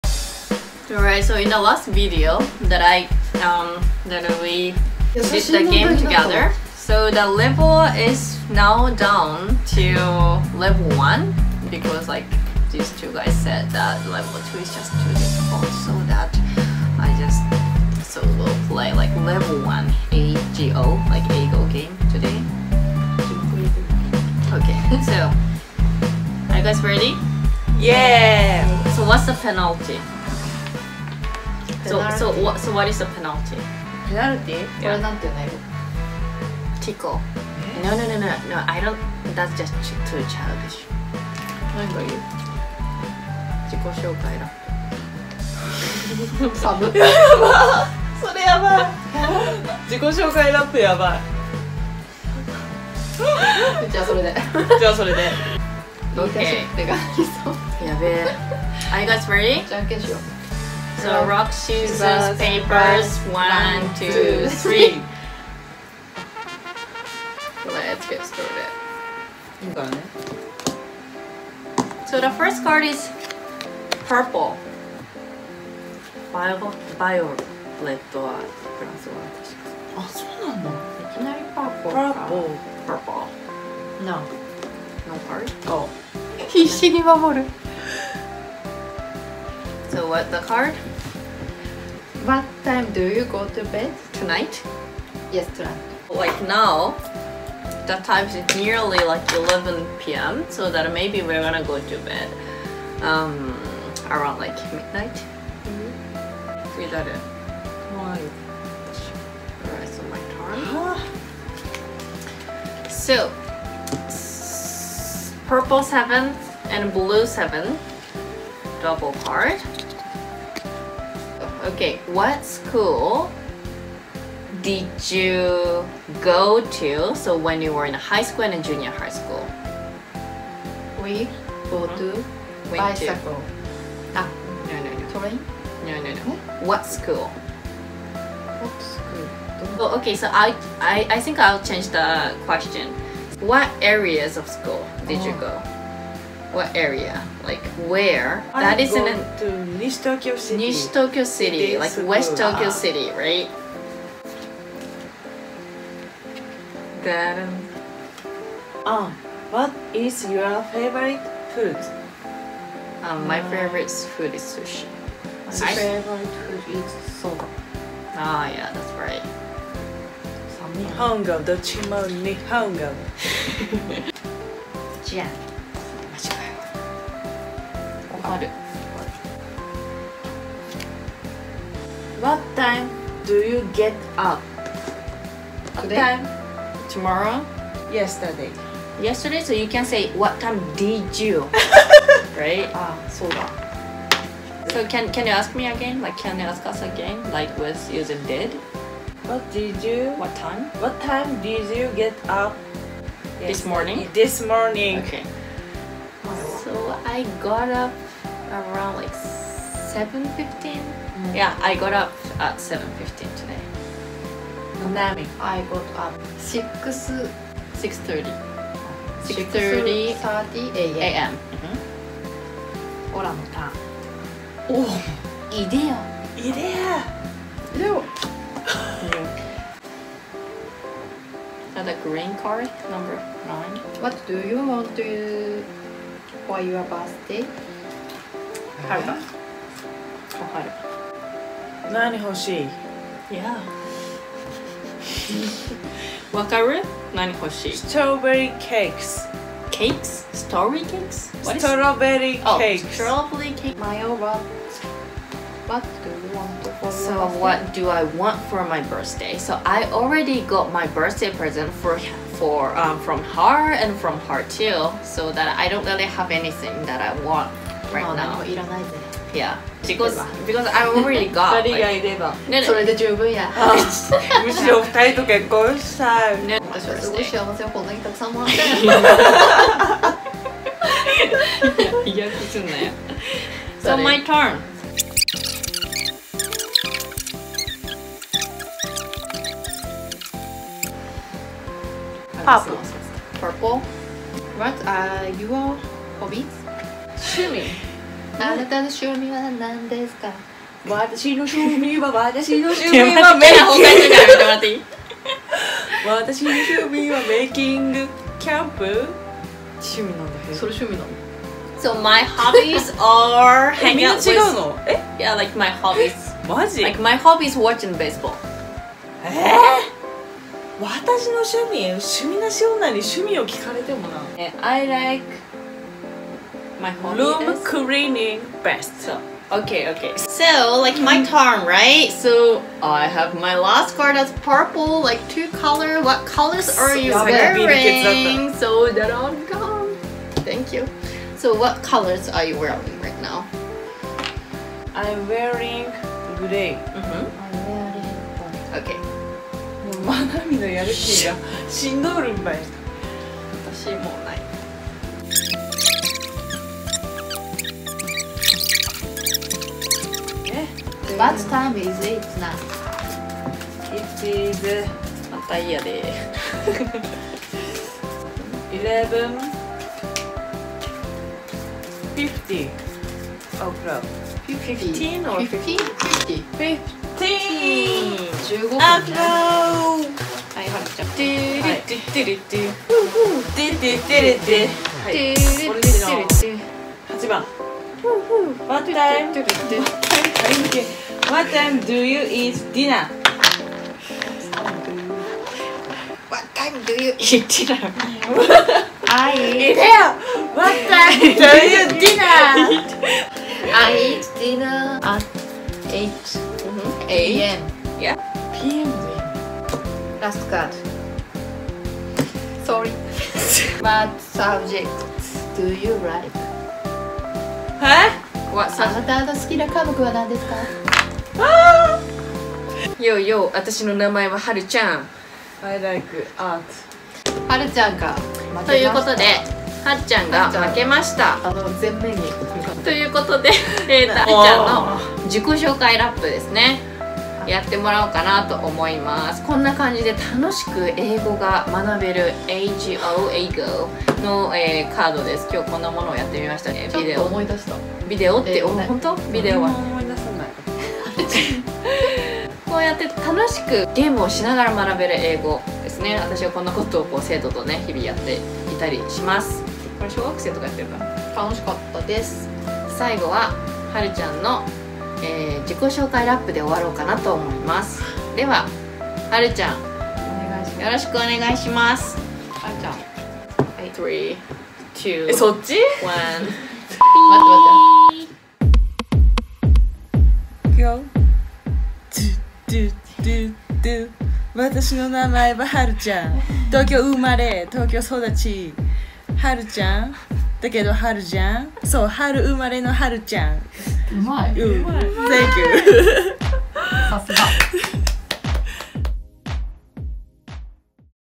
英語を楽しく学べるカードゲーム「AGO」を大人がやってみた【久々の英語】
いきなりの英語カードゲームの様子です

因みに2部構成だったつもりが、データが吹っ飛んでしまい後半部分だけ流します

先月の動画です